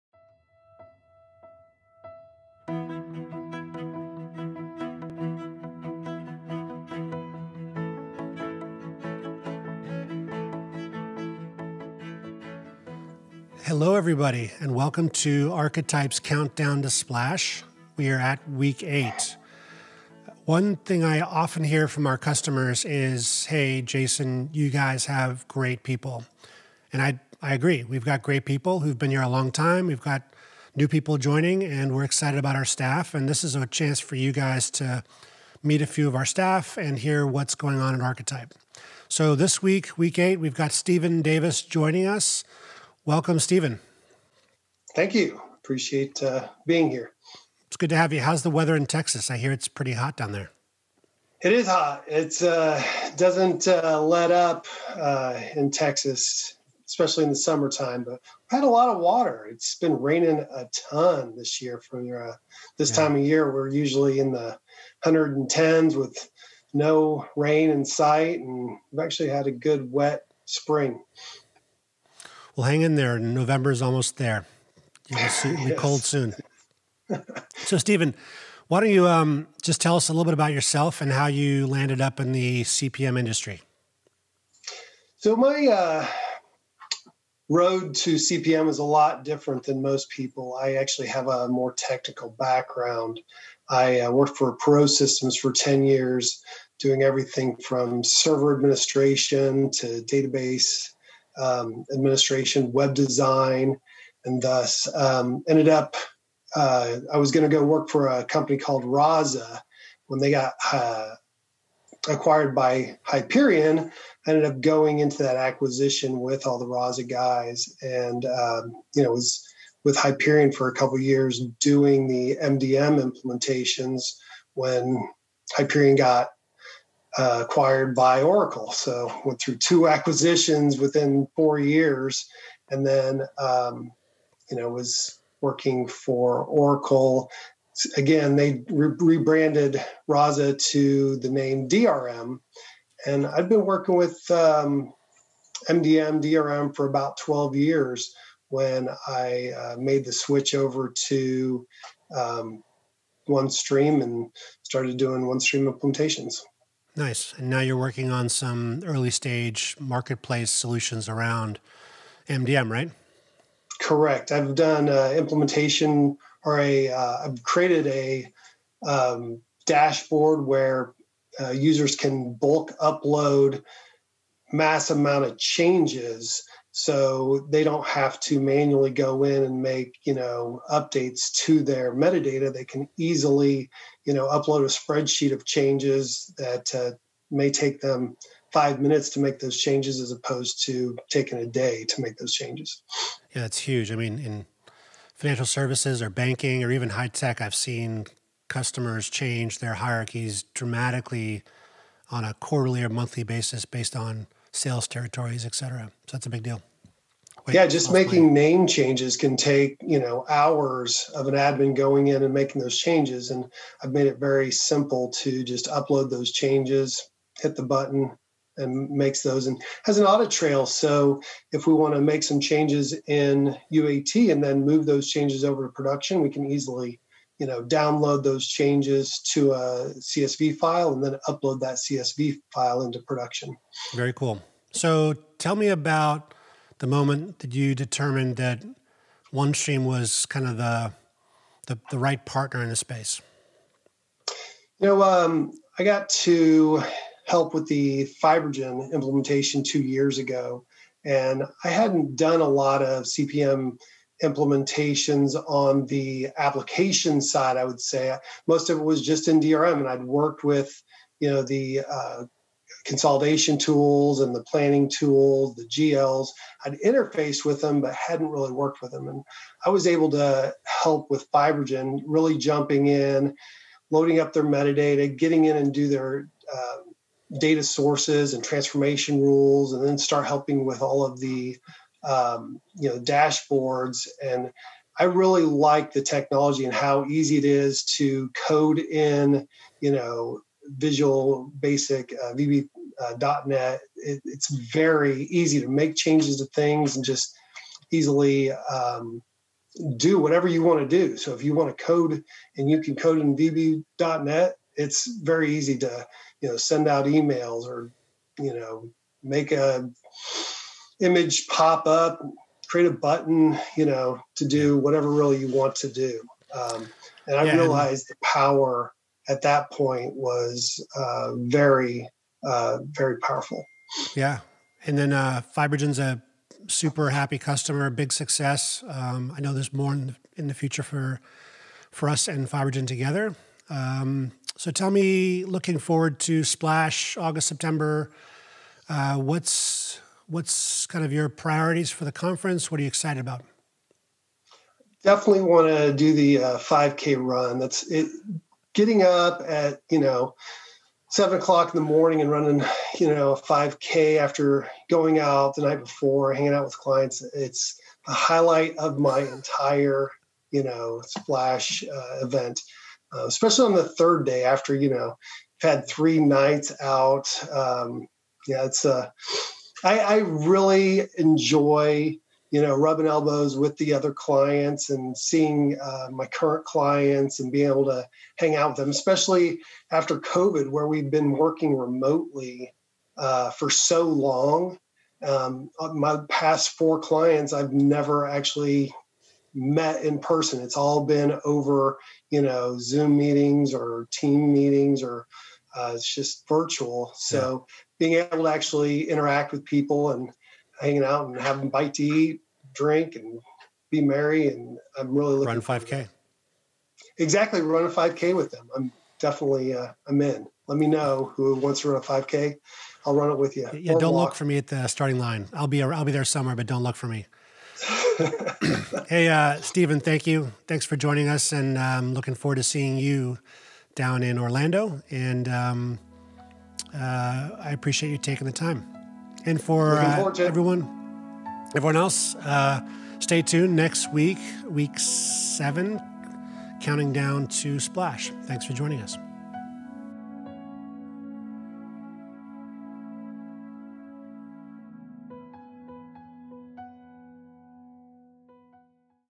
8. Countdown to SPLASH (Jason Webster & Steven Davis)
We are 8-weeks away from SPLASH in Orlando!  And if you can't tell we are extremely excited!  Each week we will be sharing a new countdown video with our Founder & CEO, Jason Webster, interviewing one of our team members who will be at Splash this year.

Make sure you register for Splash and where it says "How did you hear about this event?", make sure you put OneStream Partner - Archetype Consulting.

Tune in next week for our next countdown video and we cannot wait to see all of you at Splash this year!